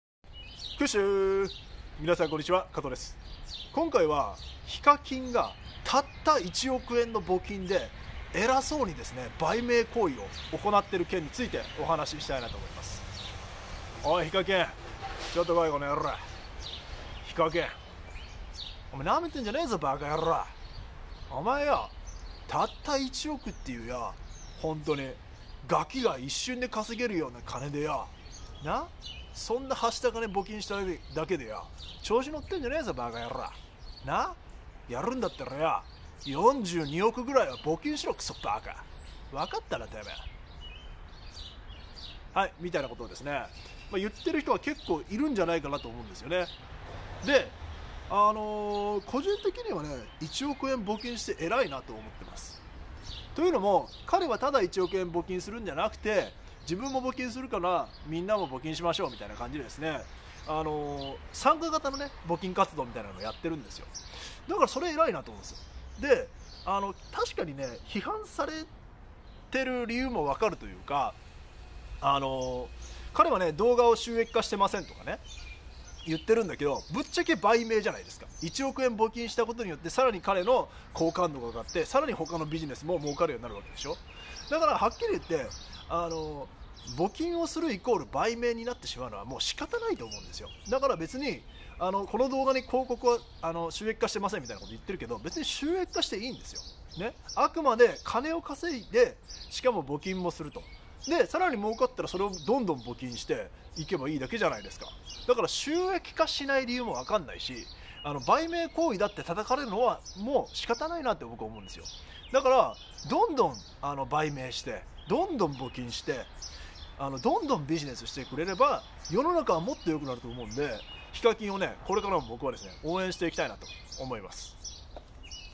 ヒカキンがたった1億円の募金で調子に乗ってる件について
おいヒカキン！調子に乗ってんじゃねーぞ馬鹿野郎！！！！俺が説教してやるからちょっと来い！！！！！！！！！！！！！！！！！！！！

※コメディー上の演出を真に受けないでください
※KATOはヒカキンの一億円募金を高評価しています